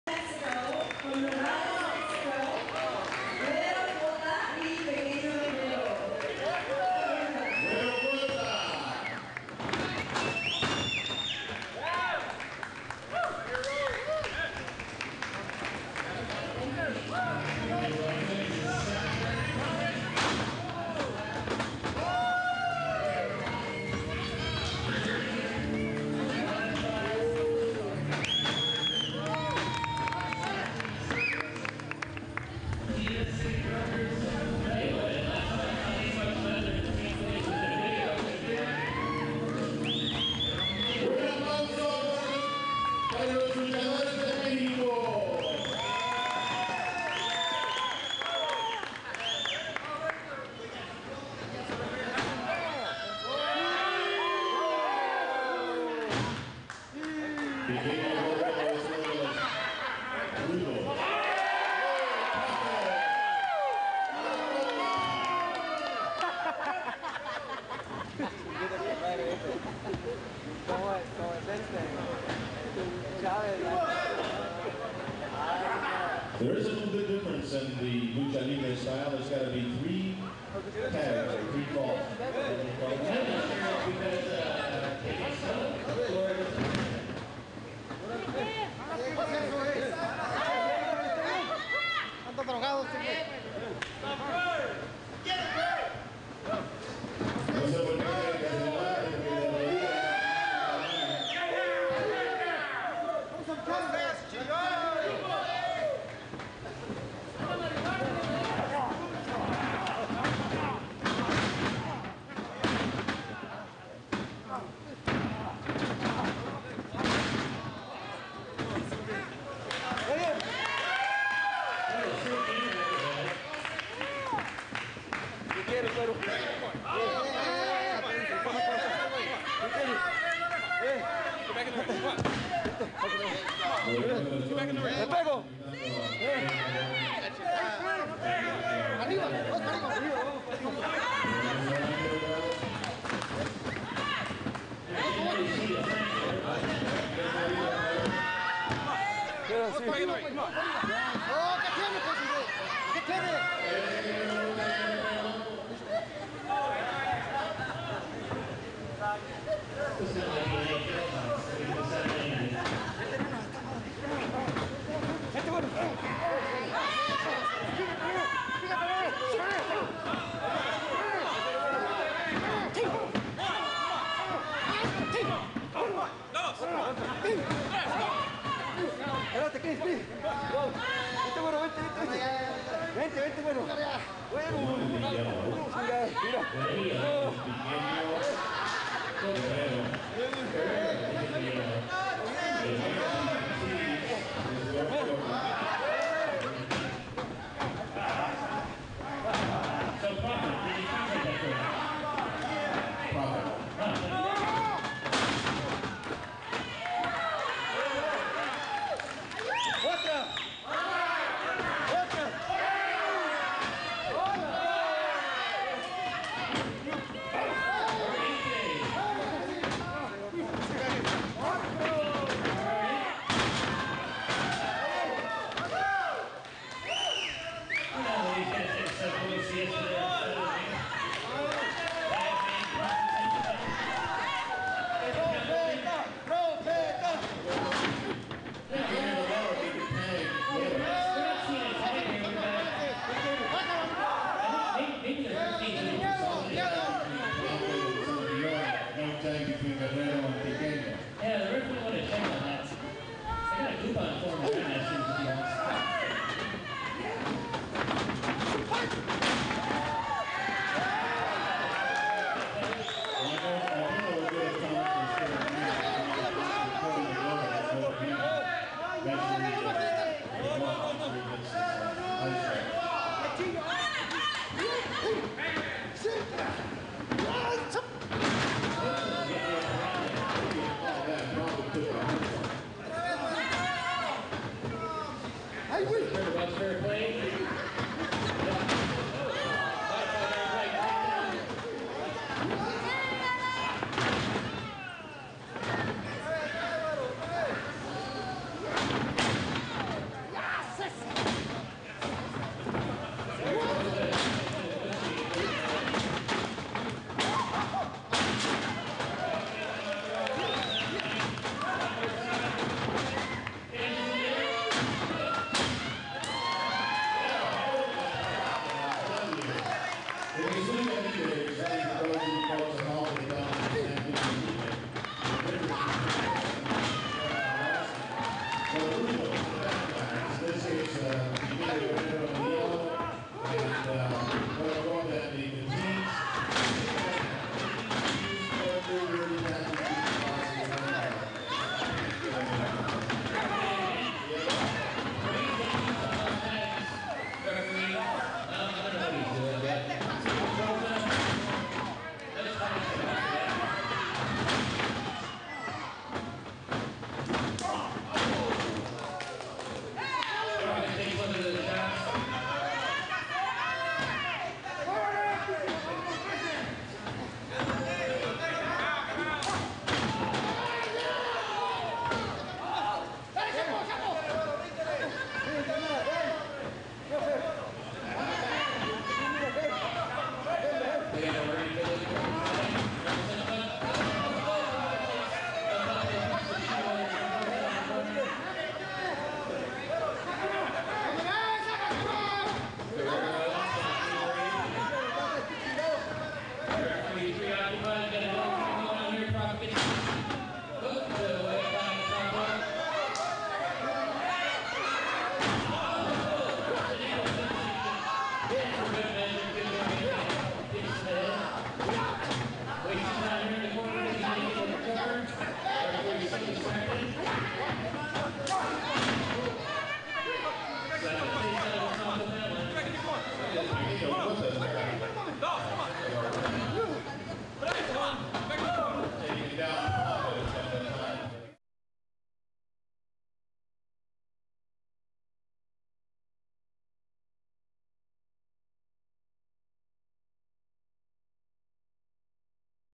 Sol De Orient Vs Guero Cota
2 Luchadores featured in the Film "Alambrista Emplumado" wrestle in this promotional event. Film comming soon from Brink DVD.